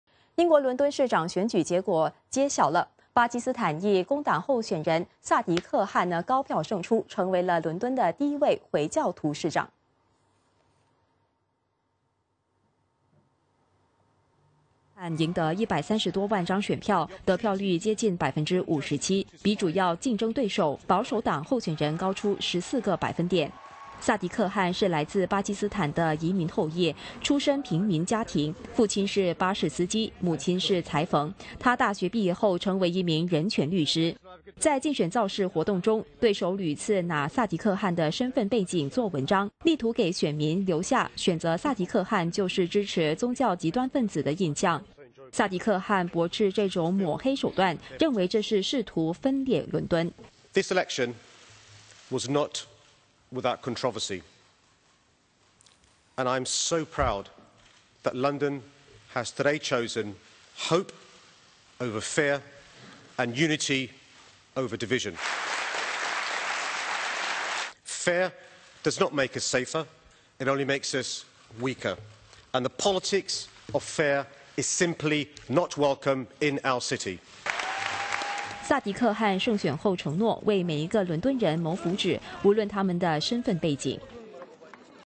英国伦敦工党候选人当选 为首位回教徒市长
英国伦敦市长选举结果揭晓，巴基斯坦裔工党候选人萨迪克汗高票胜出，成为伦敦的第一位回教徒市长。